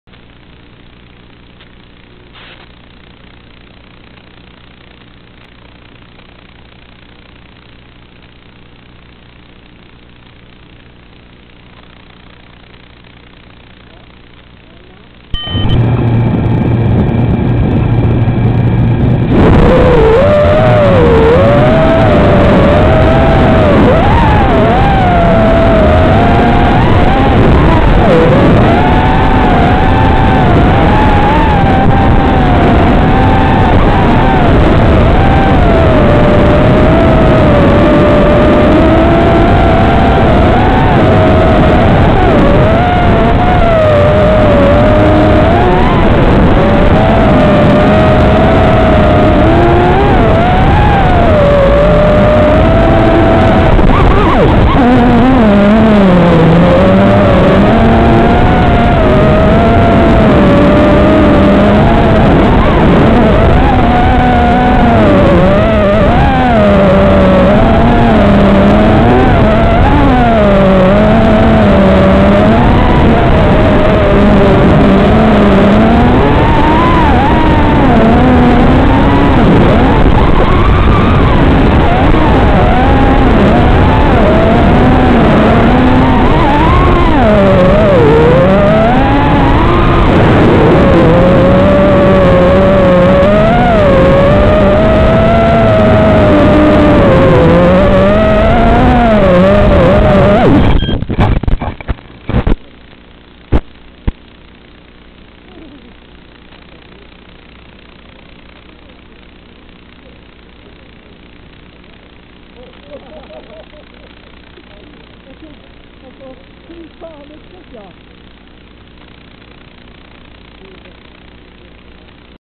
Tävling skvaden sundsvall
tappade fokus på kameran på första varvet men lyckades endå klara mig igenom alla 3 varven och kom på andra plats i det loppet.